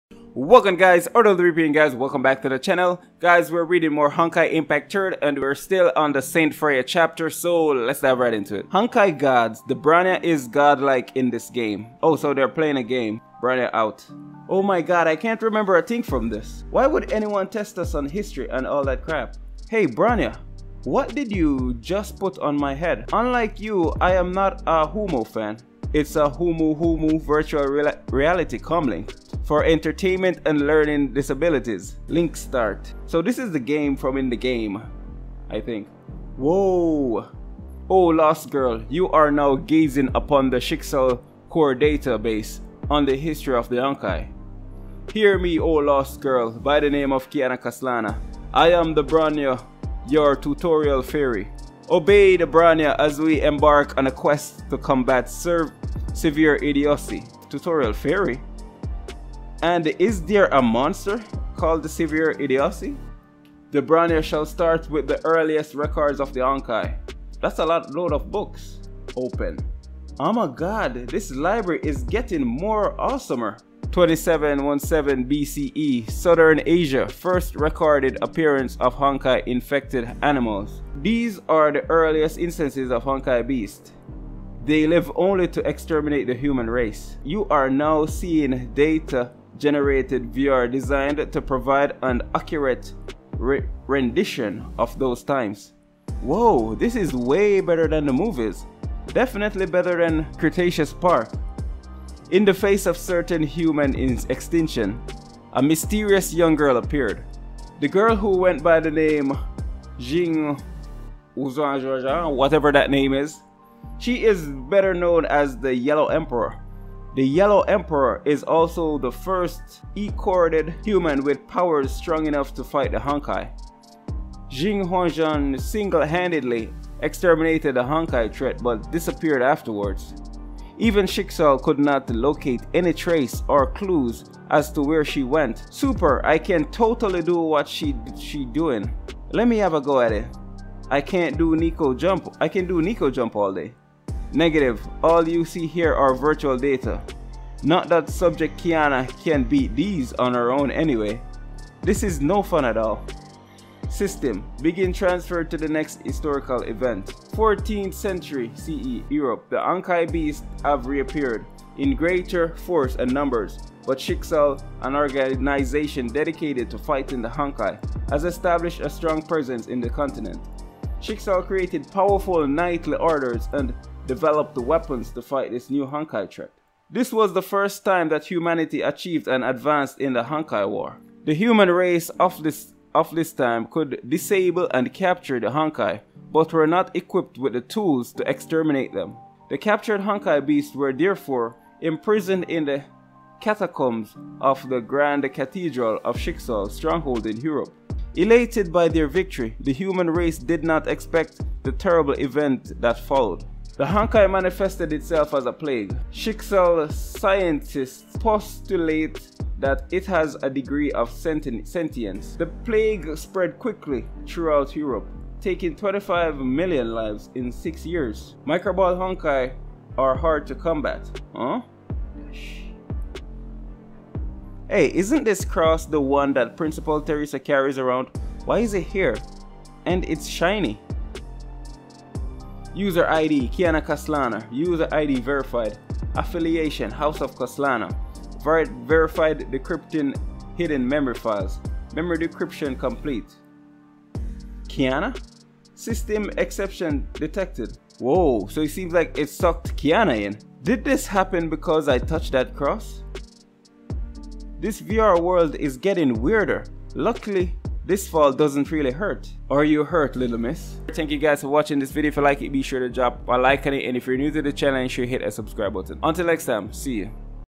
Honkai Impact 3rd - ST Freya High Manga Chapter 4 Read/ Reaction
Hey guys. In this video, I will be reading Honkai Impact 3rd - ST Freya High Manga Chapter 4.